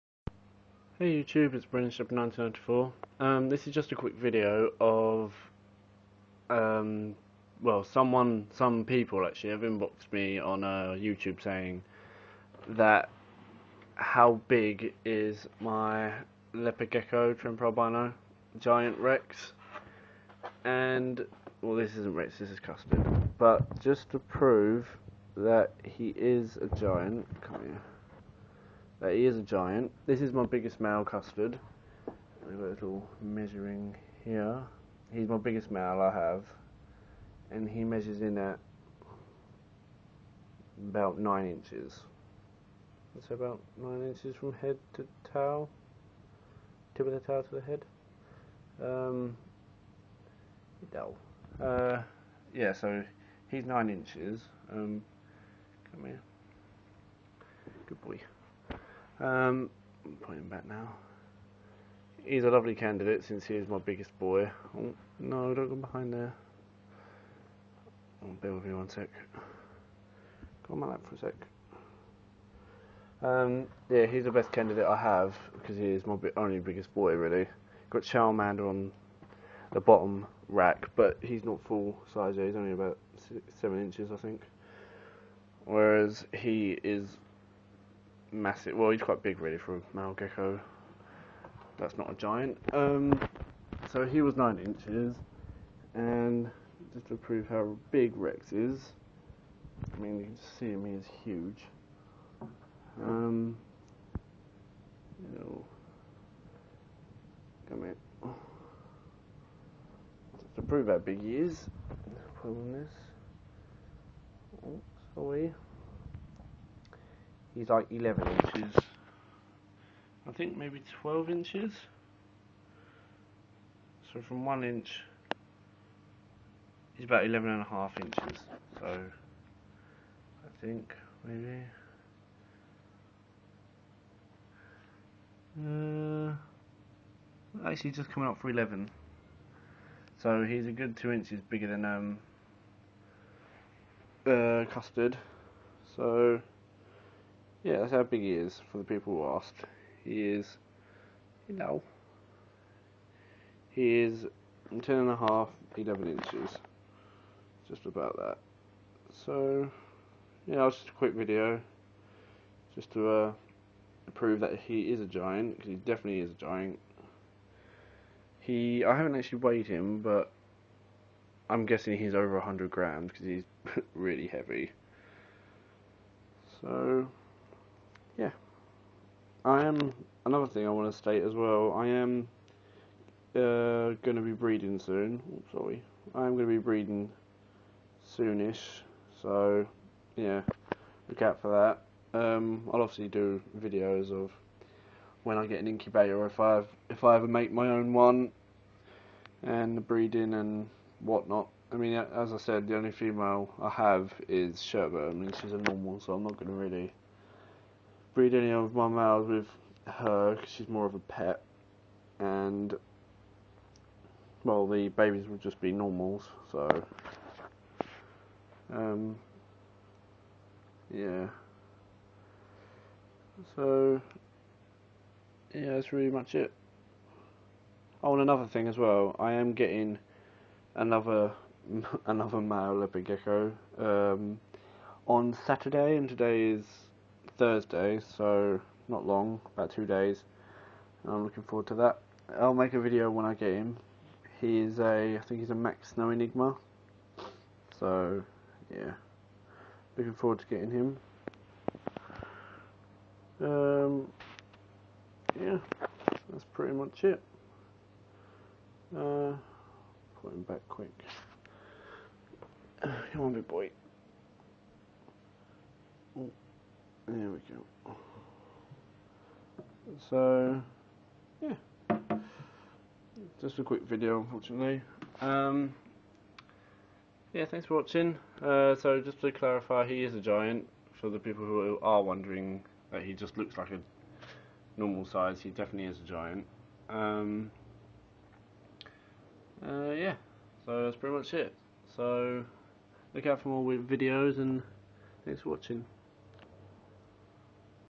Small Leopard Gecko Update
Just a quick video to prove how big Rex (My Trember Albino Giant Reverse Striped) really is compared to a standard non giant male Leopard Gecko (The candidate is Custard) Also I mention a few new things to follow up this video! :)

Comments and questions are always welcome, Please no bad comments or any abuse. Thank you and take care.